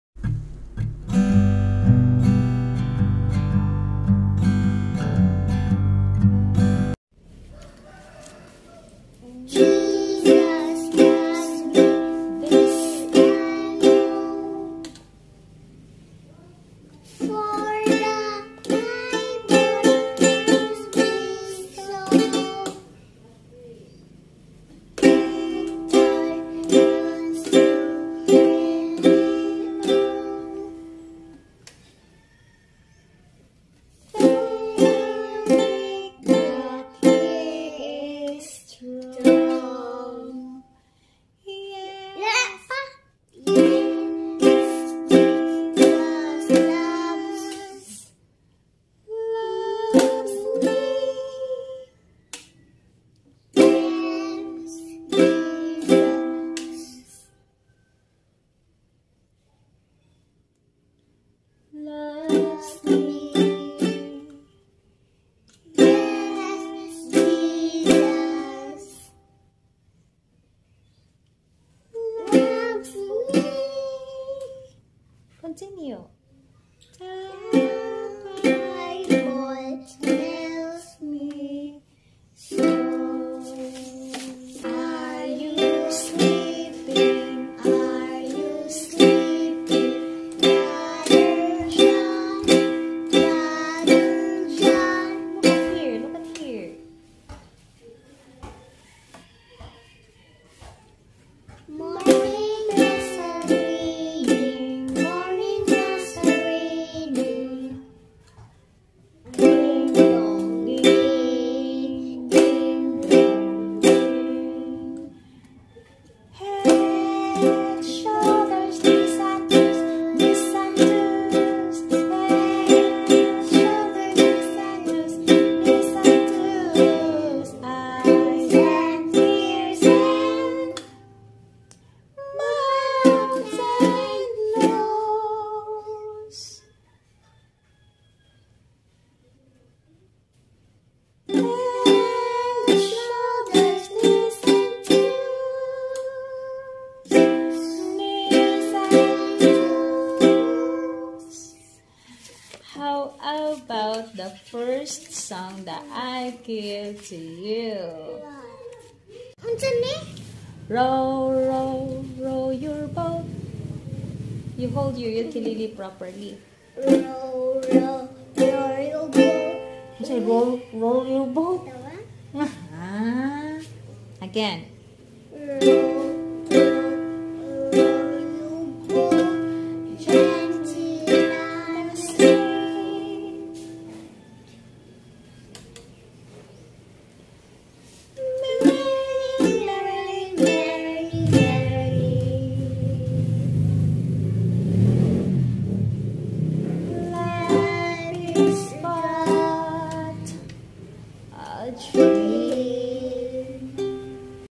Ukulele Kid Sing Along (5 Years Old Zie)
This kid learned so fast in just less than 30 minutes his 5 sessions. 😁😁😁
yeah💪😅